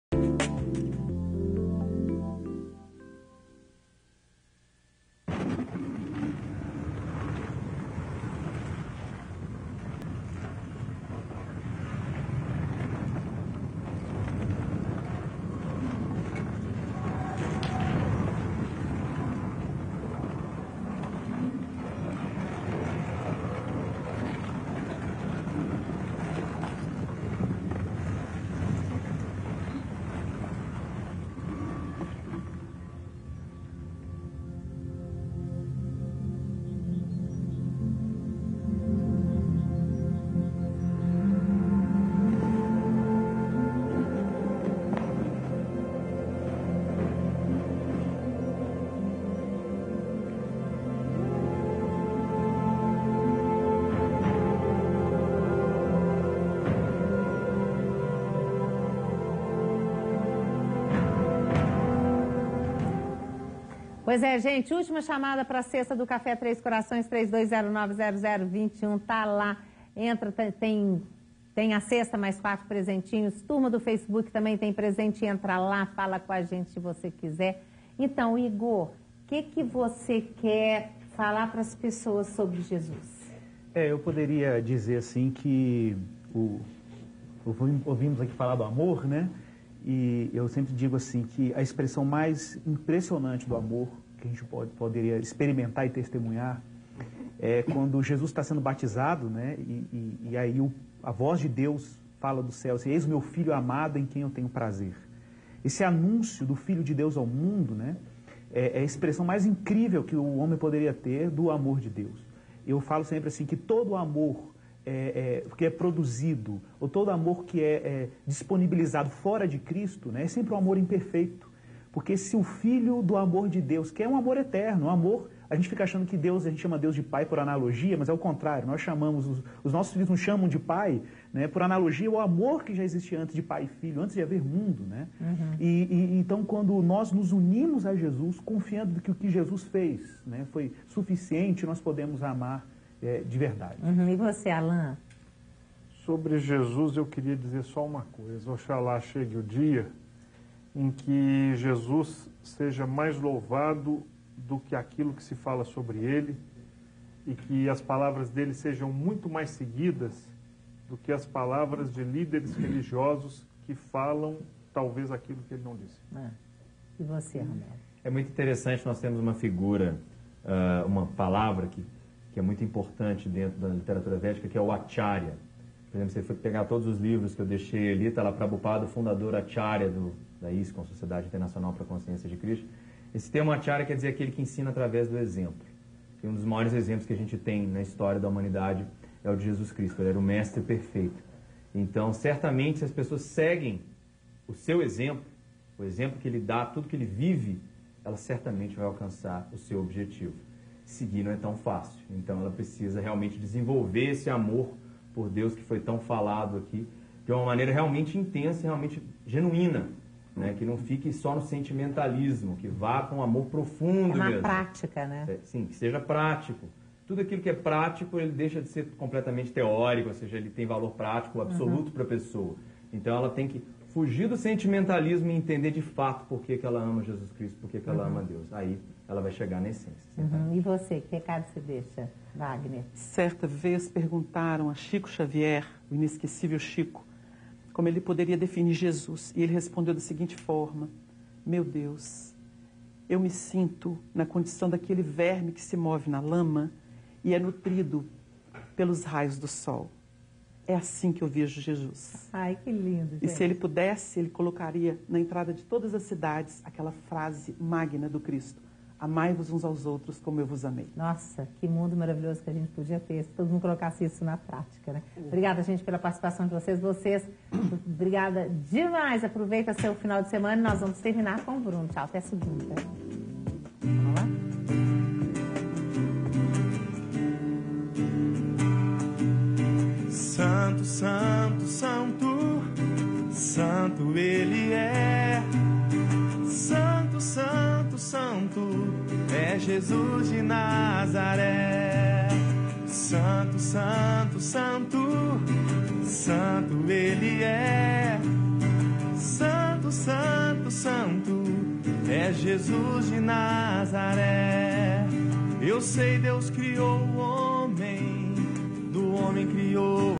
Jesus nas religiões - Parte 3
06/04/2012 - Qual é o papel de Jesus nas religiões? No Brasil das Gerais, você confere uma conversa entre um teólogo, um muçulmano, um espírita e um Hare Krishna, que falam sobre a figura de Jesus.